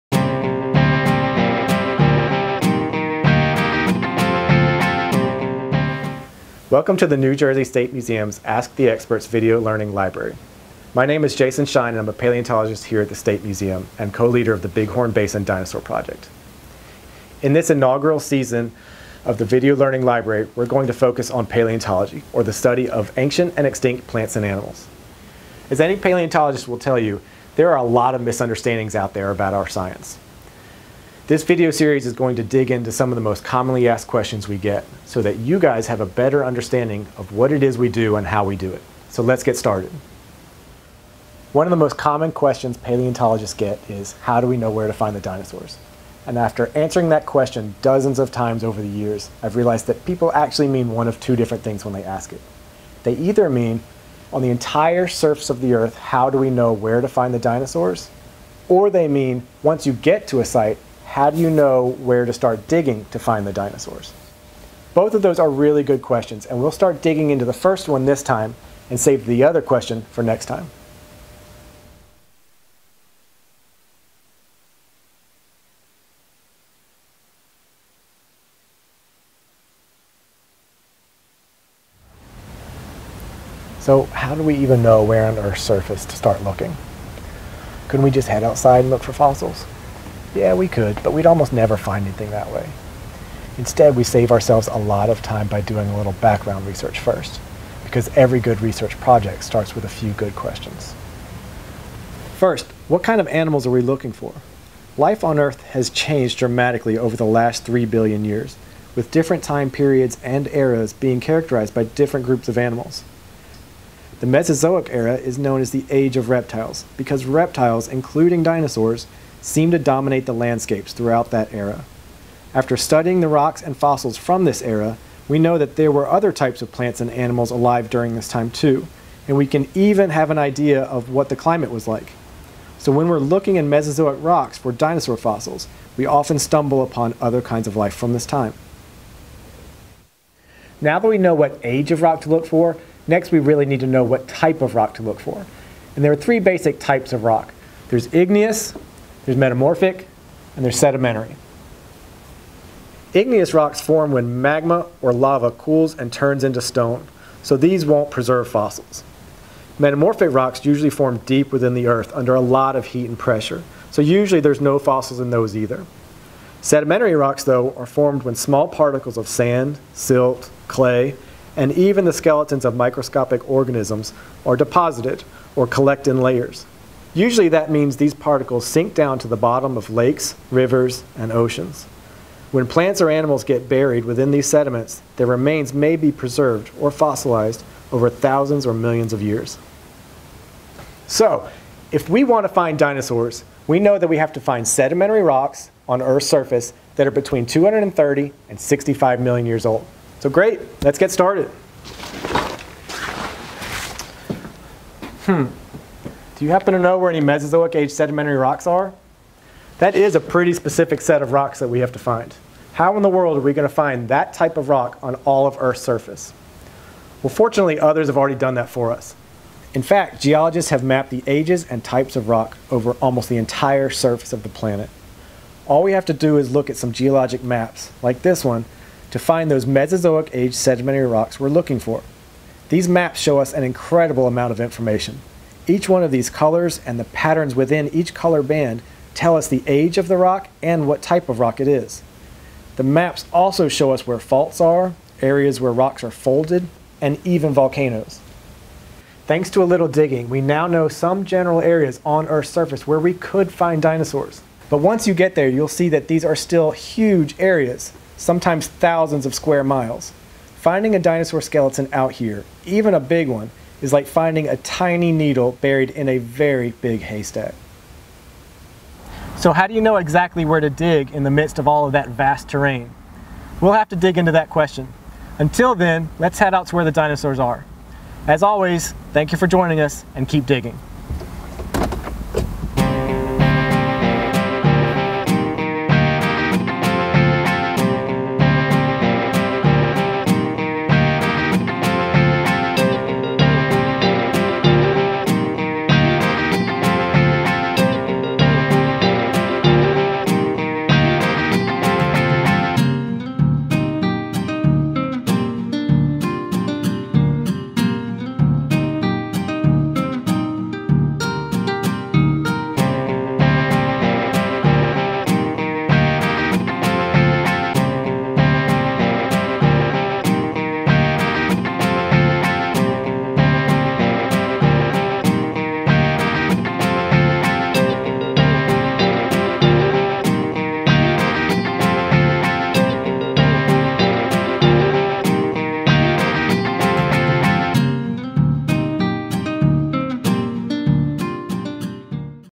Episode 1: Ask Our Experts: How Do You Know Where To Dig?
The Ask Our Experts Video Learning Library takes paleontology’s most frequently asked questions and answers them in engaging, real-life situations in the field and in the lab. Follow along with Museum Paleontologist Jason Schein, as he plans the field season, and then as he leads the Bighorn Basin Dinosaur Project’s 2015 Field Expedition in Montana and Wyoming.